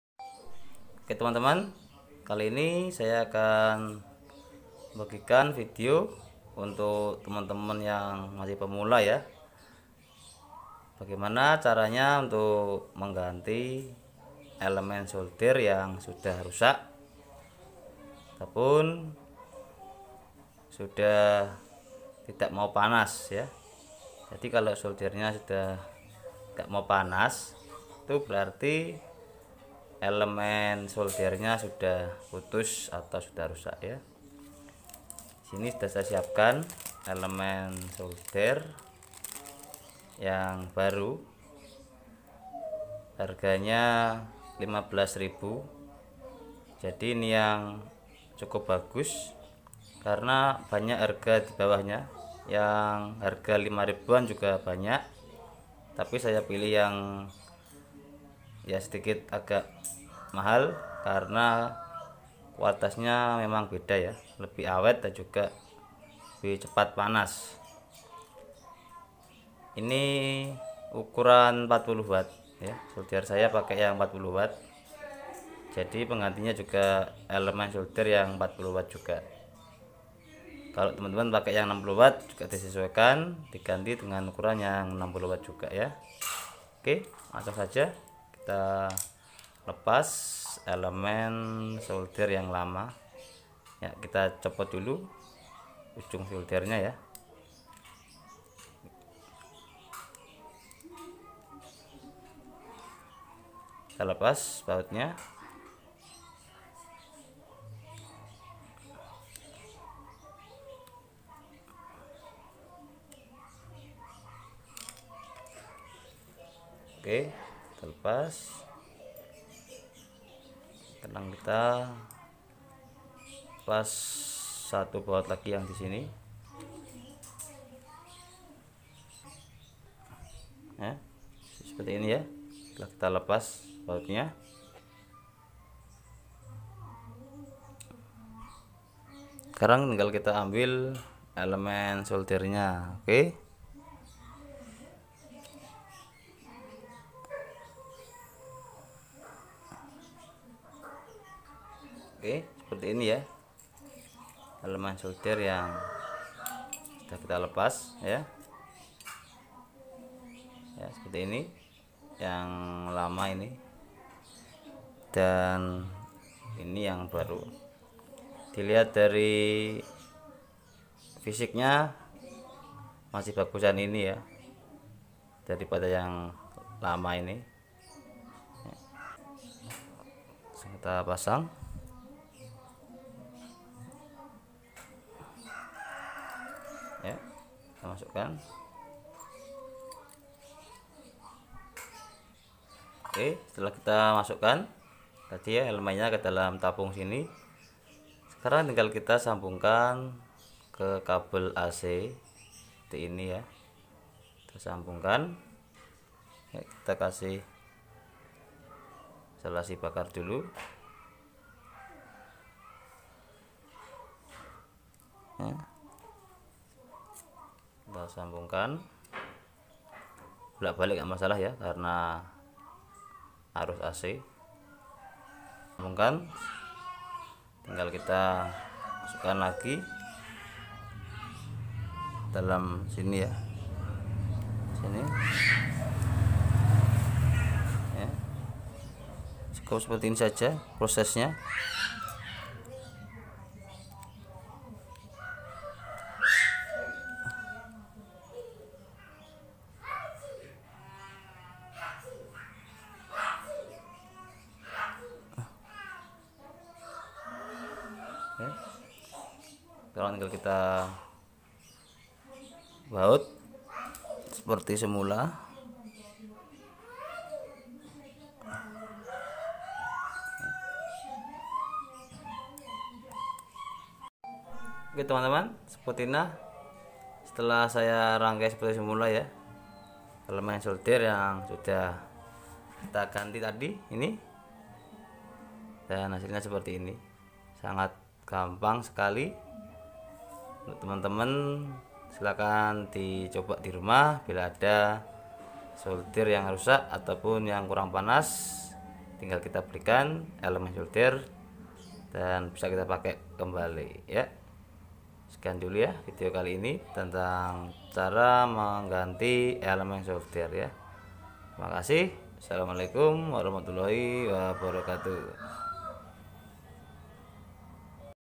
Solusi solder tidak panas || Menganti elemen solder
Buat teman-teman yang punya solder rusak atau tidak bisa panas kemungkinan bisa karena elemen solder sudah rusak atau putus.

Jadi setelah kita mengganti elemen soldernya hasilnya solder akan panas kembali seperti semula.

Untuk cara mengganti elemen solder yang putus bisa teman-teman lihat pada video ini dan silahkan di coba dirumah.

Semoga video ini bisa bermanfaat untuk kita semua.